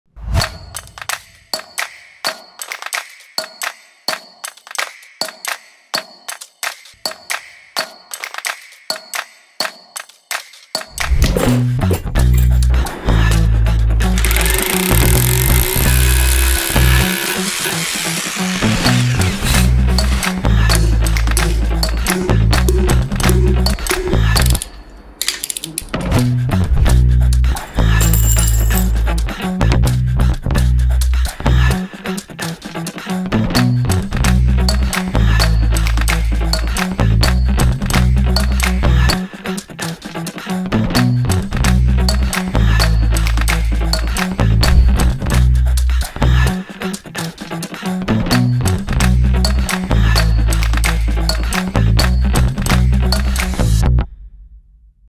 Kotwy ekspresowe LE Klimas Wkręt-met
Najwyższej jakości kotwy mechaniczne od polskiego producenta to gwarancja:
- pewności mocowania
- dużej antykorozyjności
- prostego montażu
- oszczędności czasu
- odporności ogniowej R30-R120
- Europejskich Norm ETA.